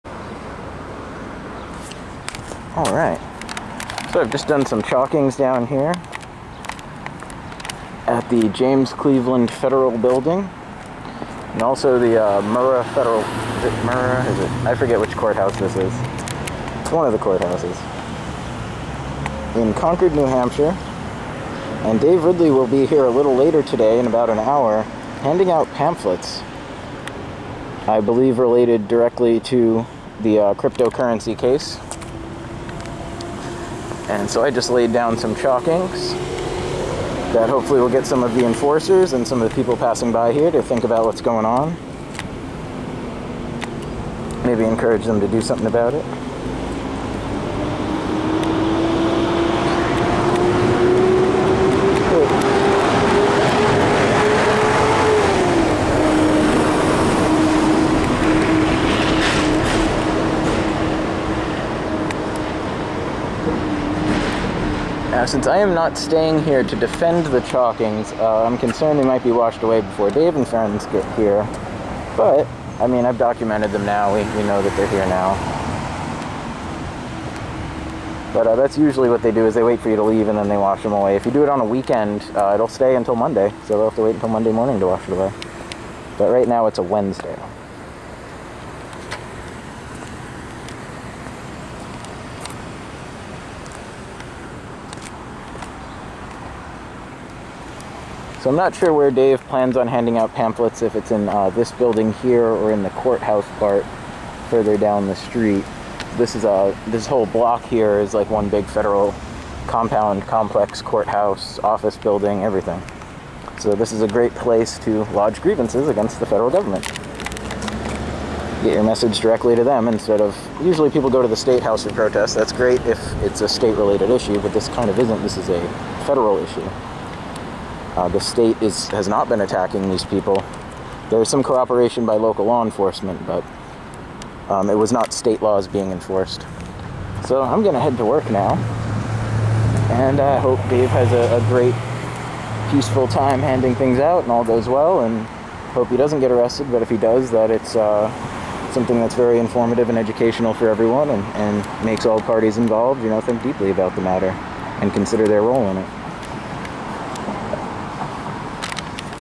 Chalking Feds re:Crypto 6 Case, 6/2/21
James Cleveland Federal Building - Concord NH
Garret chalks the sidewalks around the federal compound prior to a civil disobedience event by Dave Ridley as Bitcoin Gandhi. Chalking relate to the release of victimless criminals from confinement by federal authorities. Chalkings read, No Victim, No Crime, Life in Prison for Crypto Sales is Morally Abhorrent, Good People Disobey Bad Laws, and Good Cops Don't Enforce Bad Laws.